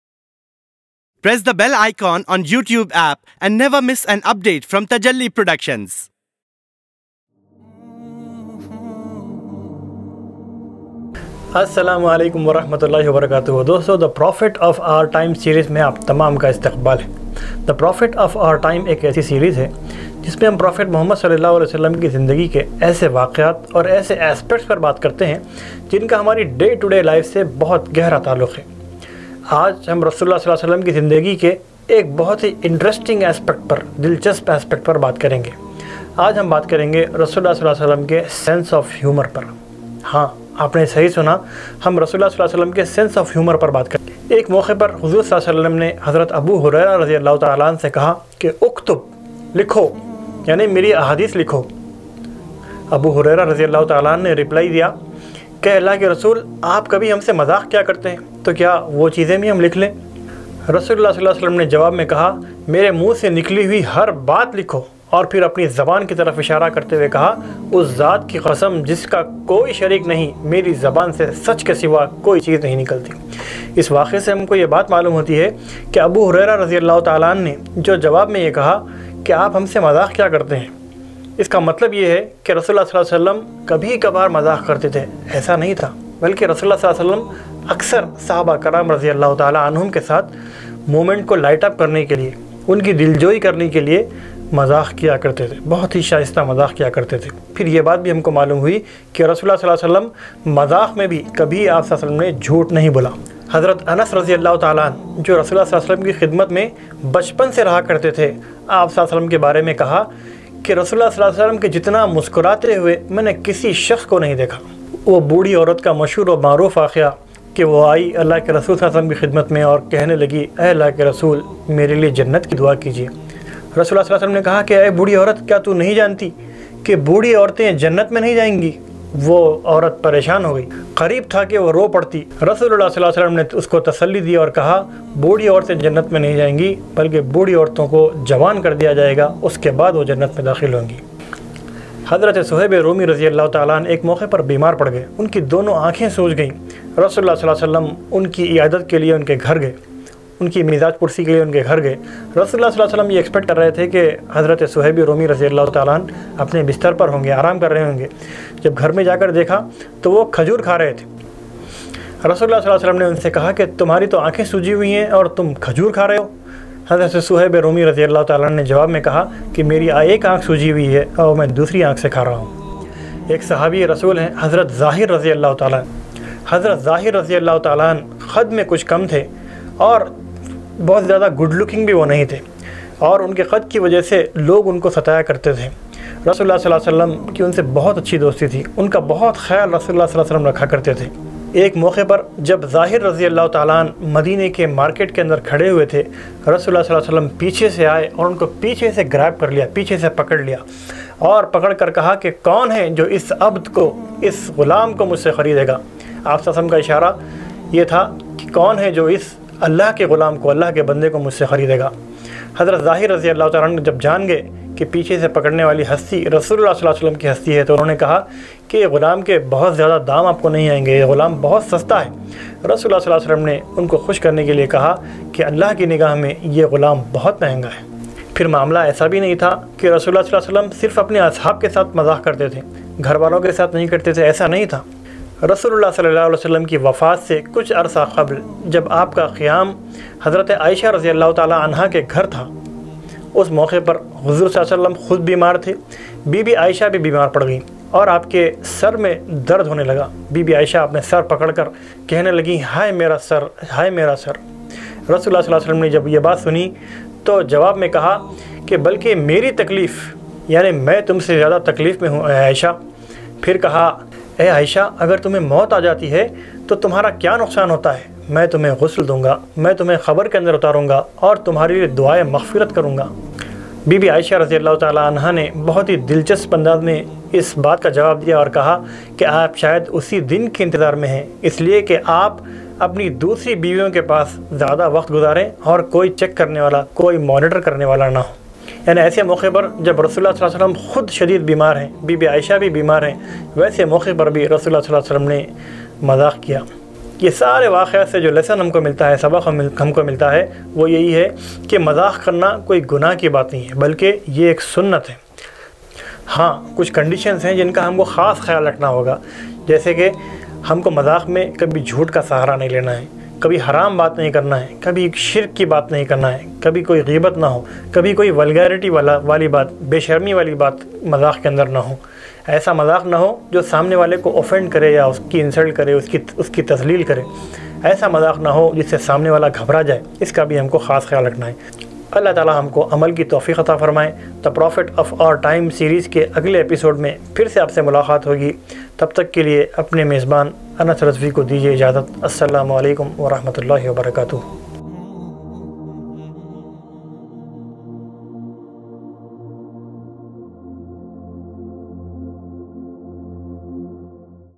Prophet Muhammad's (PBUH) Sense of Humour | Short Talk (5Min)
Prophet Muhammad's (PBUH) Sense of Humour

This short talk by our host, Br. Anas Razvi comprises of authentic stories from the life of Prophet Muhammad PBUH. These stories are few of the many occasions Prophet Muhammad PBUH light up the moment by being humorous with his companions and family.

This is part of Tajalli's short talk series titled "The Prophet of our time".